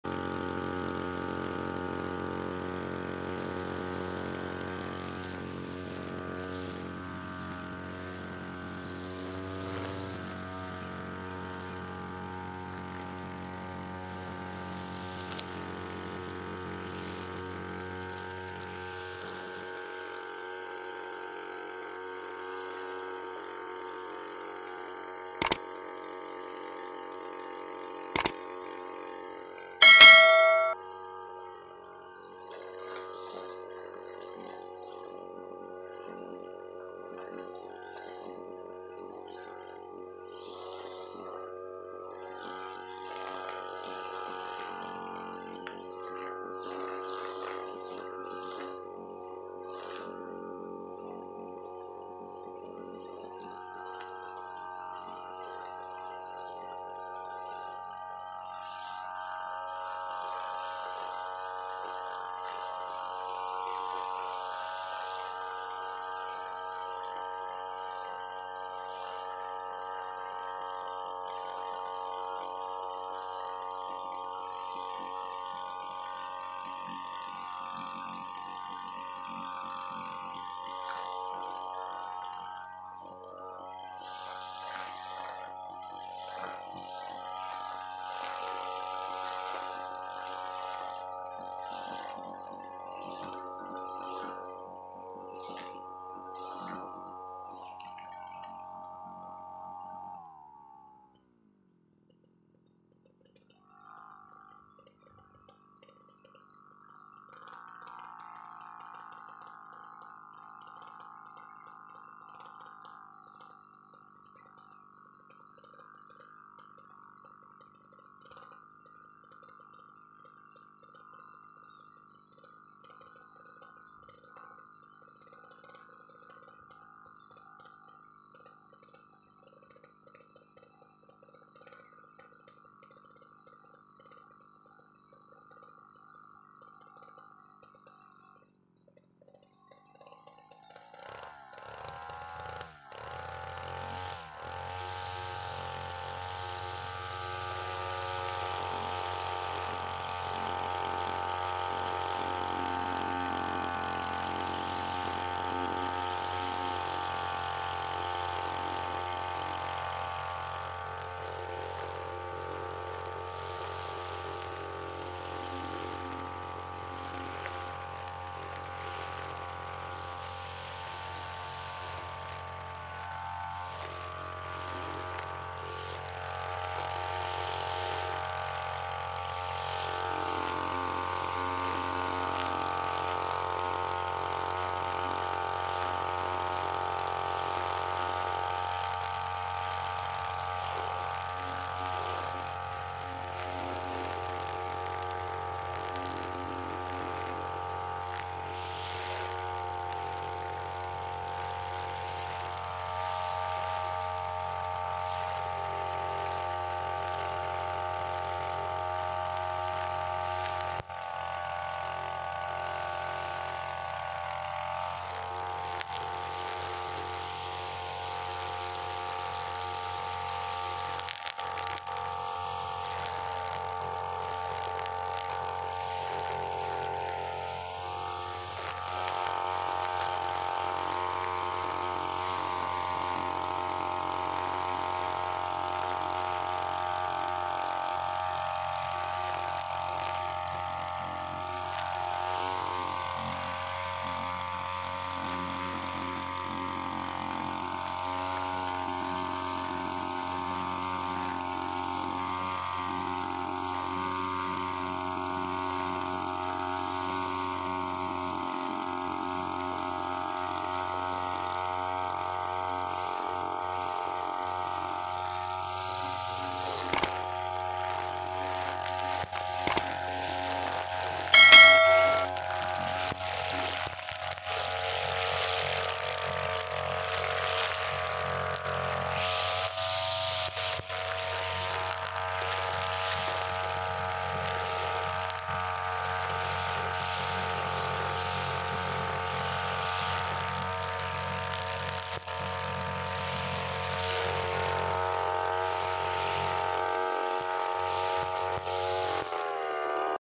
Part 37~Tebas rumput sampai tanah ~ guna tali senar bergigi 3.0mm ~ UM328 Brush cutter machine
Menebas rumput sampai tanah, uji coba tali senar bergigi pada rumput yang liat. Semoga Allah member rezeki yang melimpah limpah kepada kalian.